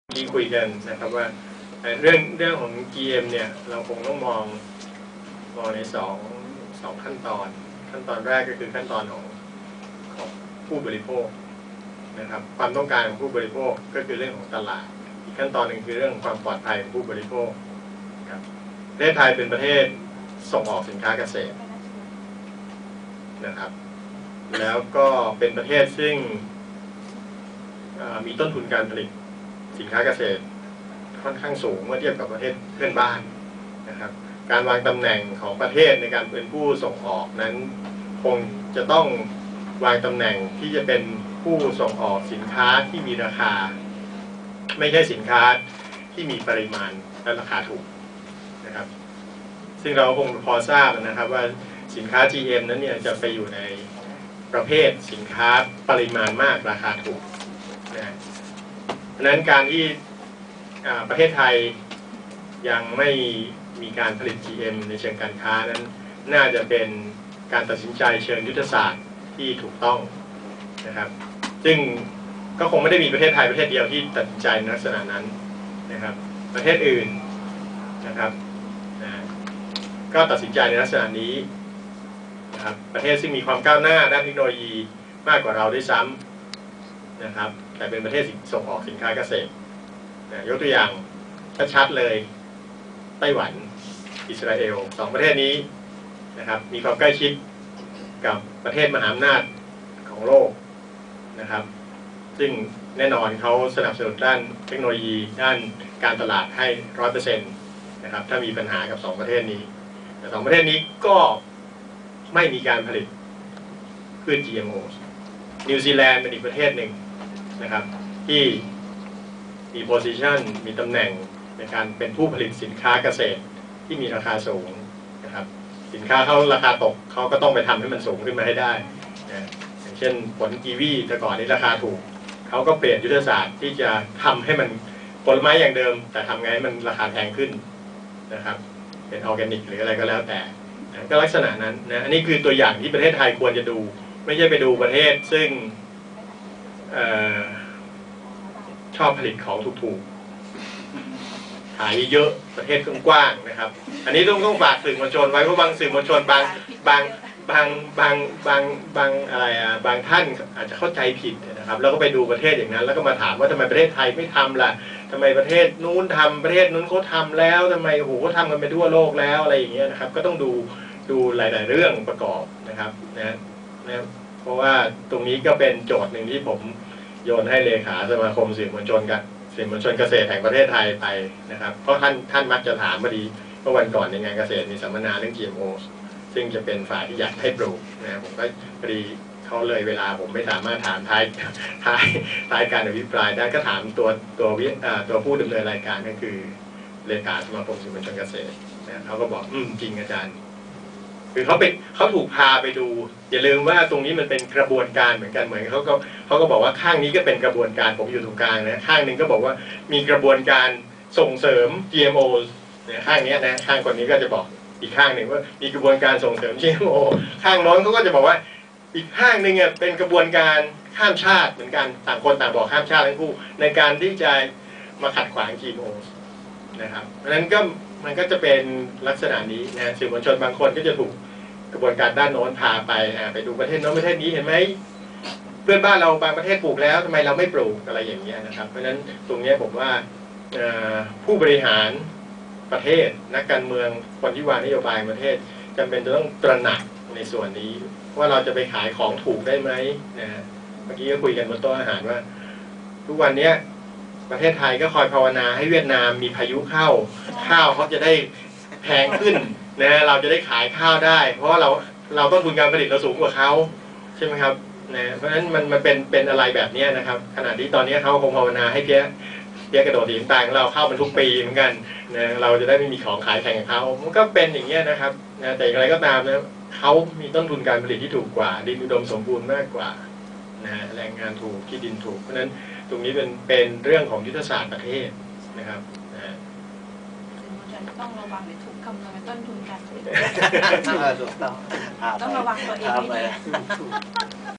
ยุทธศาสตร์เกษตรกรรมไทย.flv
รศ.ดร.สุรวิช วรรณไกรโรจน์ อาจารย์ประจำคณะเกษตร ม.เกษตรศาสตร์  เสนอว่า การผลิตของเราต้องมองความต้องการและความปลอดภัยของผู้บริโภค  ทั้งตลาดในประเทศ และในฐานะผลิตและผู้ส่งออกสินค้าที่มีคุณภาพ ราคาดี ยุทธศาสตร์ที่ไม่เอาไม่ผลิตจีเอ็มโอเป็นพืชเศรษฐกิจน่าจะถูกต้อง ซึ่งเป็นไปในทิศทางเดียวกับประเทศที่ก้าวหน้าในการส่งออกสินค้าเกษตร เช่น อิสราเอล ไต้หวัน และนิวซีแลนด์ ซึ่งไทยไม่ควรไปแข่งการผลิตอาหารไร้คุณภาพ ราคาถูก และมีปัญหาในการผลิตมากมายตามมา ที่มักมีการพานักข่าวไปดูงาน (8 ก.พ. 53)